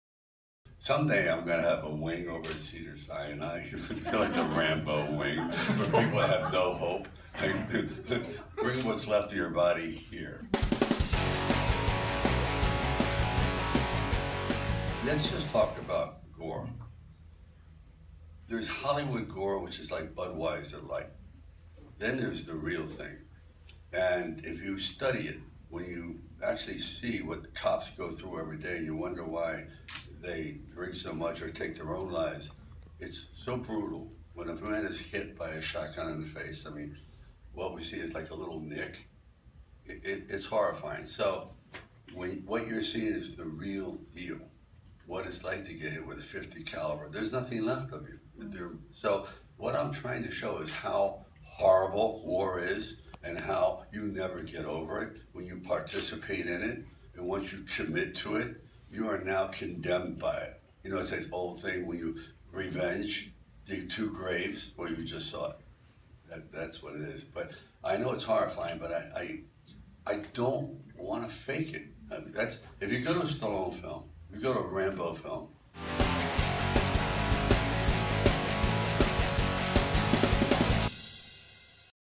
Sylvester Stallone On Why You'll See Real Gore and Not Hollywood Violence in Rambo Last Blood
You expect violence when you're at a Rambo movie, but Sylvester Stallone says they amped it up for LAST BLOOD. The action star says he wanted the bloody gore to be real rather than Hollywood violence.